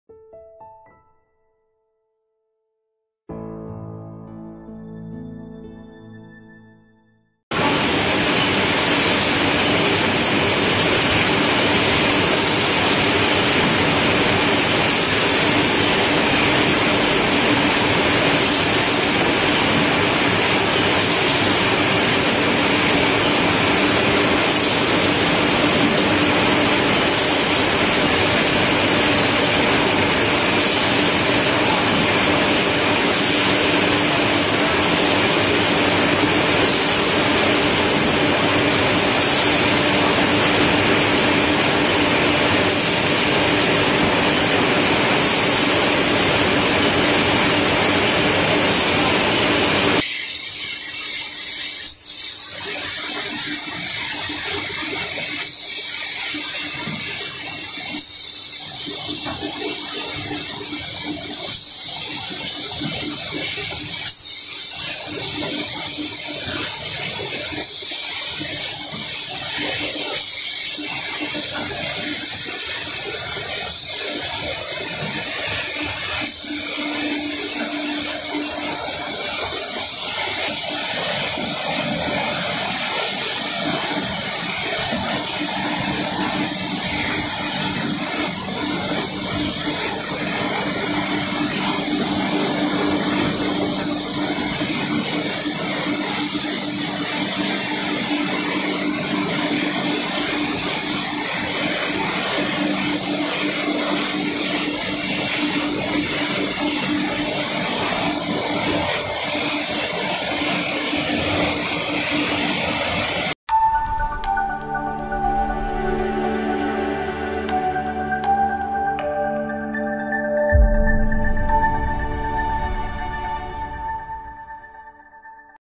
LC MB6535
Upper and Lower Film Sealing& Cutting Heat Shrink Packaging Machine
Specially designed for beer, beverage, drinking water, juice, dairy,etc.